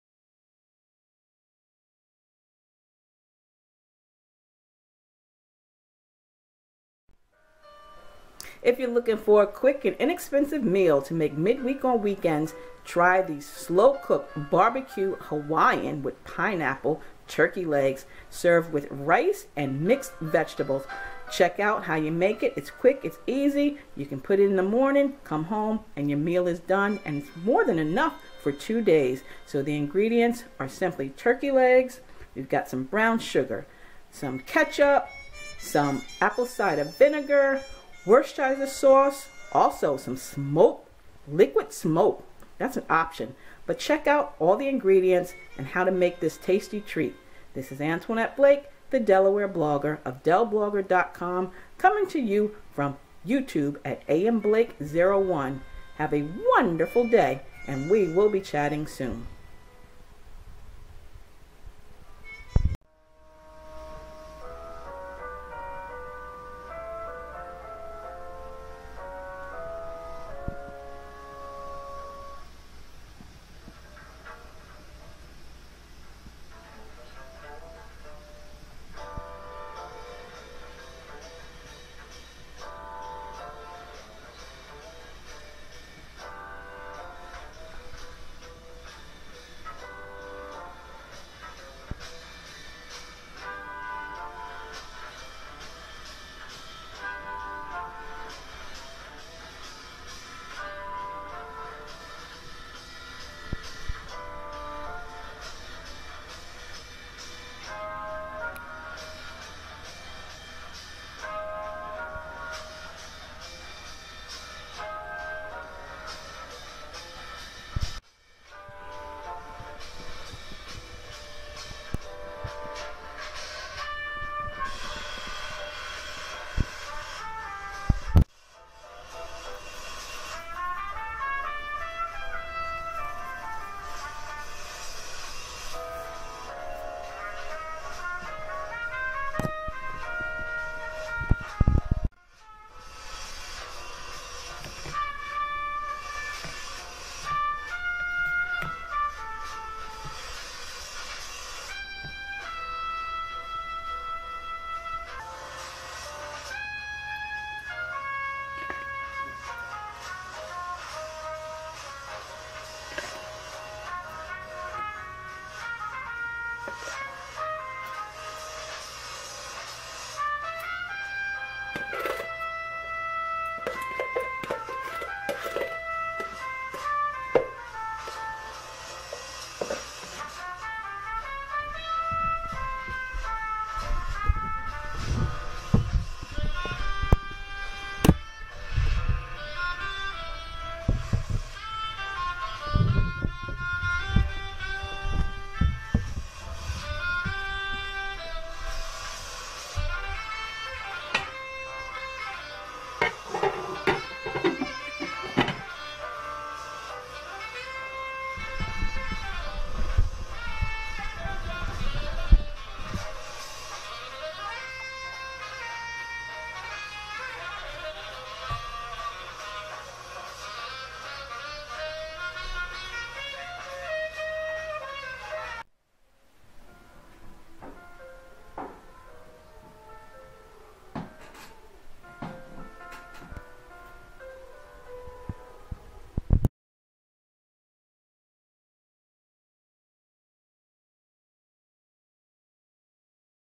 Slow Cooked BBQ Turkey Legs
Turkey Legs slow cooked in the Crock Pot